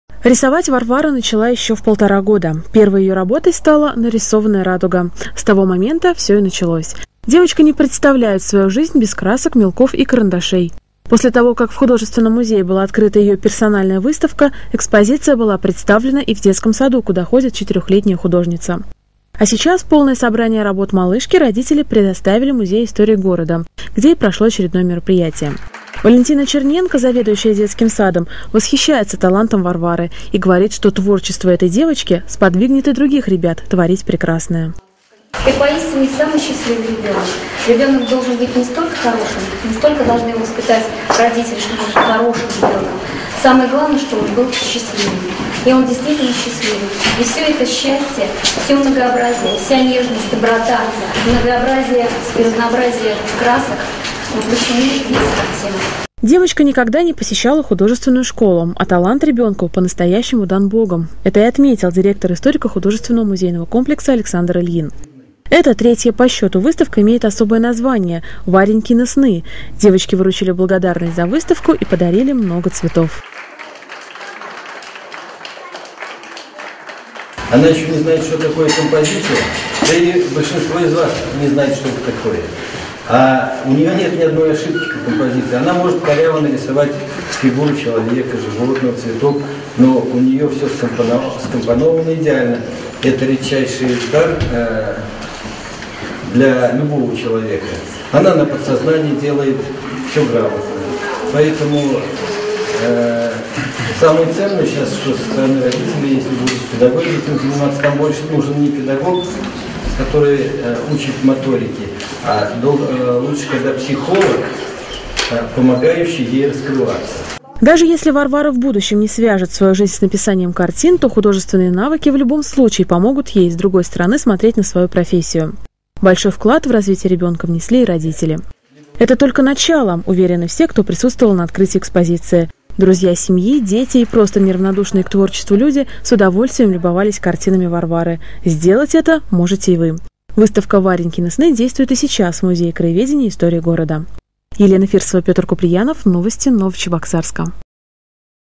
В музее истории города в 3 раз открылась персональная выставка рисунков юной горожанки Варвары
На днях в Новочебоксарске, в музее истории города открылась персональная выставка рисунков юной горожанки Варвары Гусельщиковой. Подобное мероприятие проходит в городе уже в третий раз.